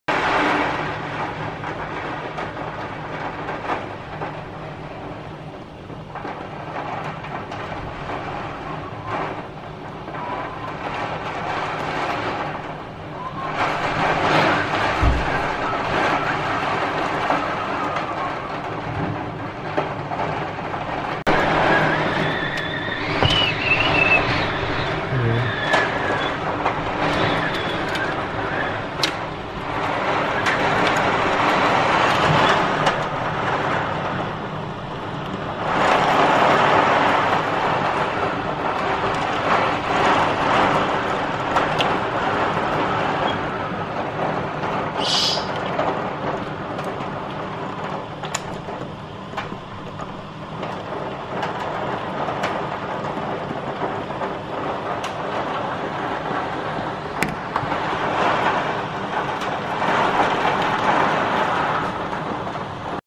Today. Storm, Heavy rain with hail in Kiev - Резкое изменение погоды! Ливень и град.Киев
After the exhausting heat, the weather in Kiev has changed dramatically! This afternoon the strong wind was blowing , and the heavy rain with hail was falling!

¡Gracias por su atención, Amigos!
¡Hasta luego!